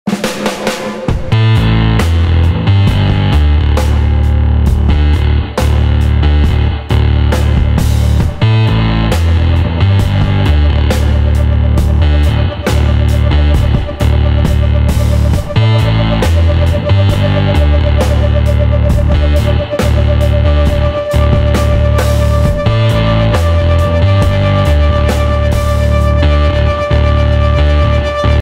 Precise Detail Pros First Cars & Coffee event!!
This past Sunday on 1/12/2020 we hosted our very first Cars & Coffee event. Thank you to everyone who came out. We had several more bad ass rides stop by that I didn’t get to video. Our next one is in February!